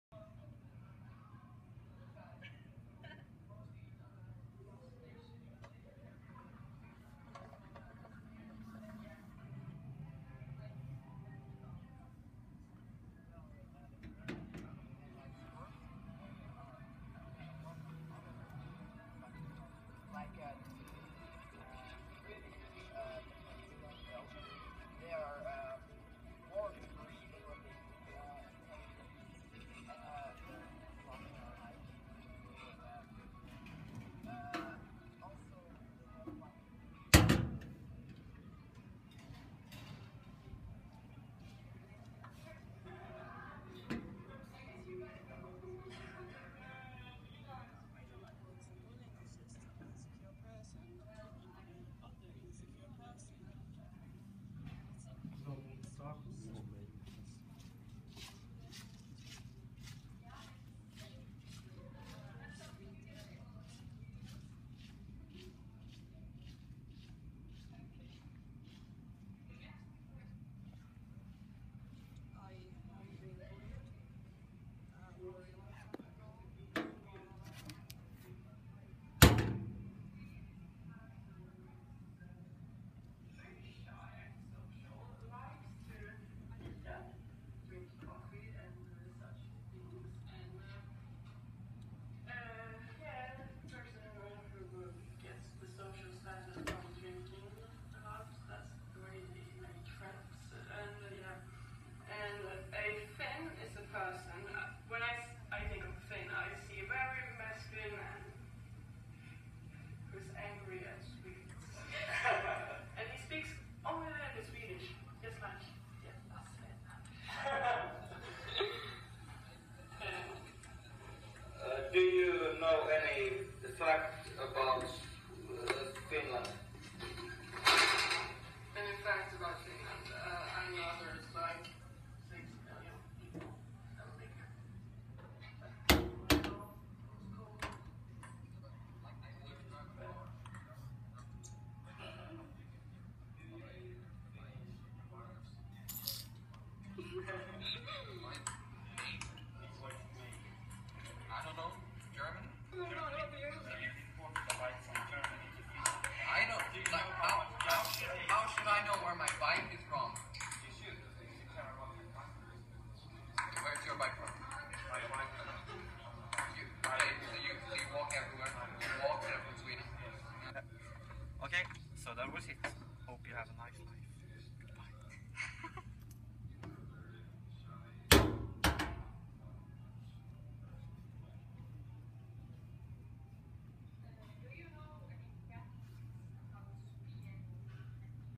DIG_IT digital art in Finland
Students created "locker art", made videos and decorated the locker to represent their theme somehow. They studied themes related to stereotypes or issues that they found somehow relevant.